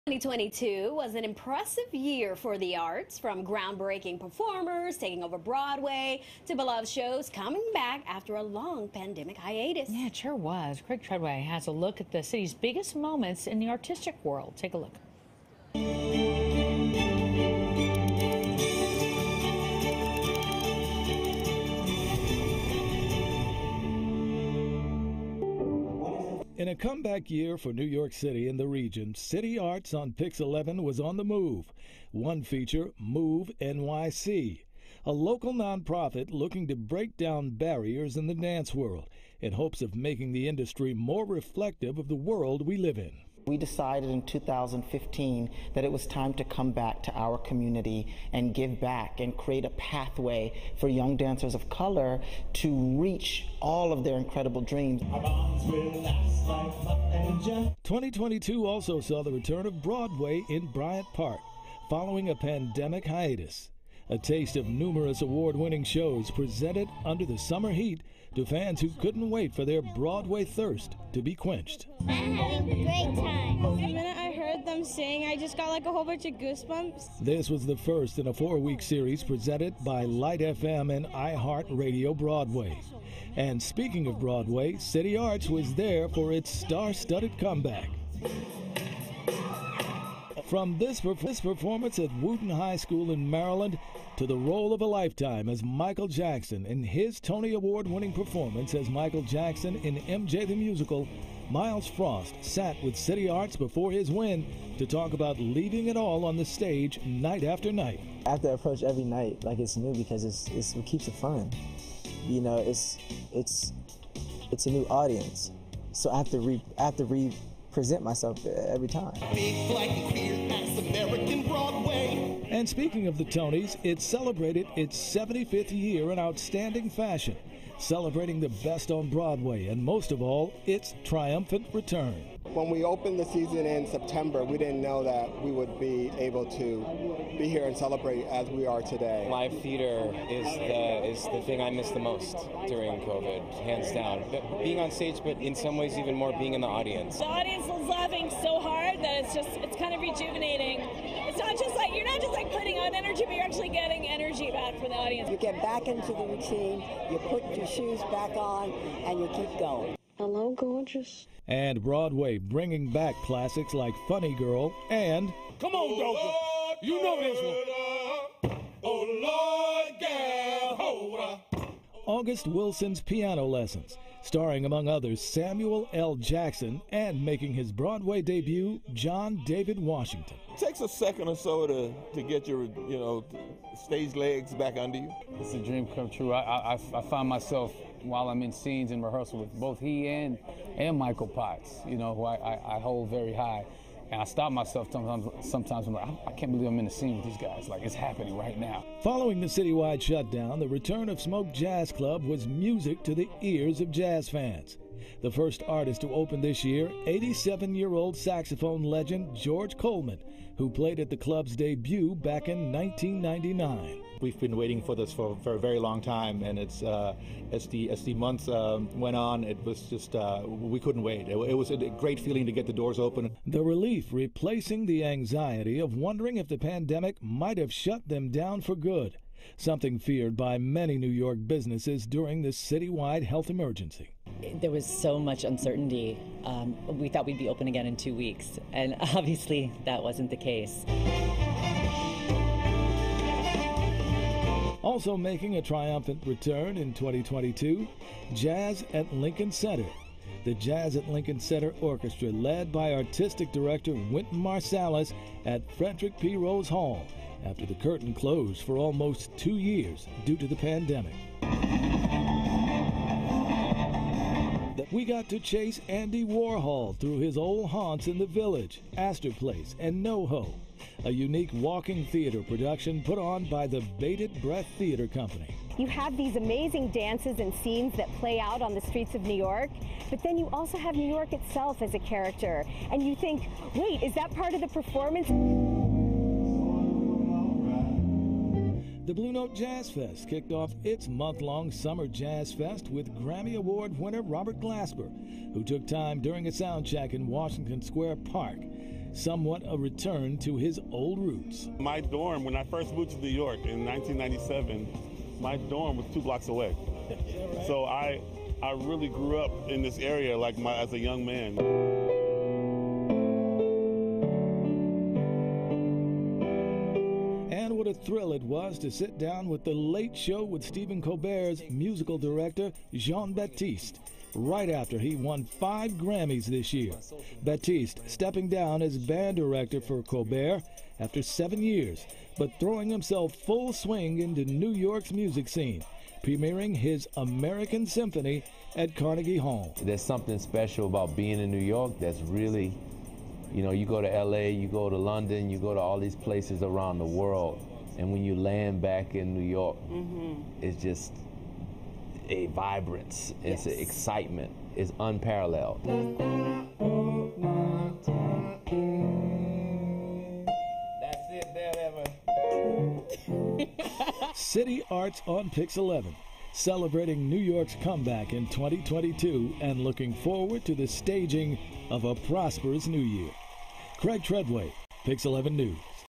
2022's biggest artistic moments
PIX11's Craig Treadway looks back on big moments in music, art, theater and more from 2022.